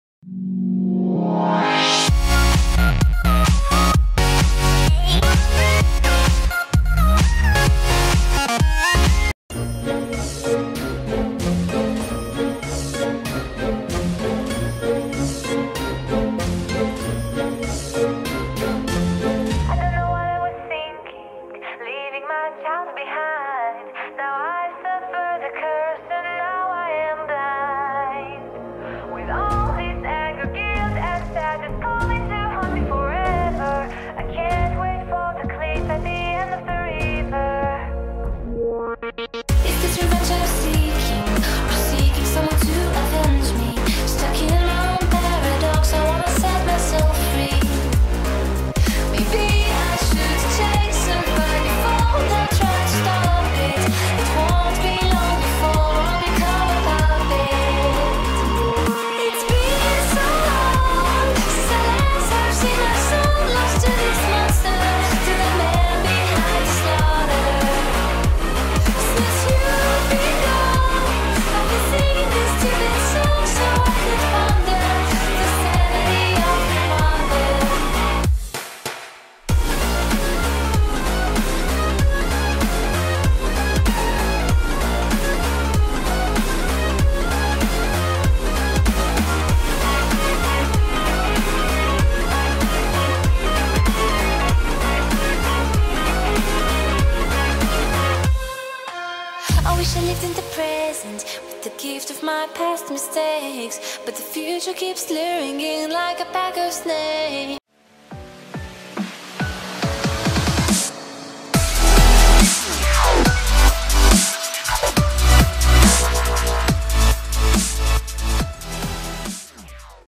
My take on Dietstabs Five Nights At Freddys Animation
Whats up guys uber here back with another extended video on my take on dietstabs five nights at freddys video.Anyway i hope you guys all like it and i will see you guys later.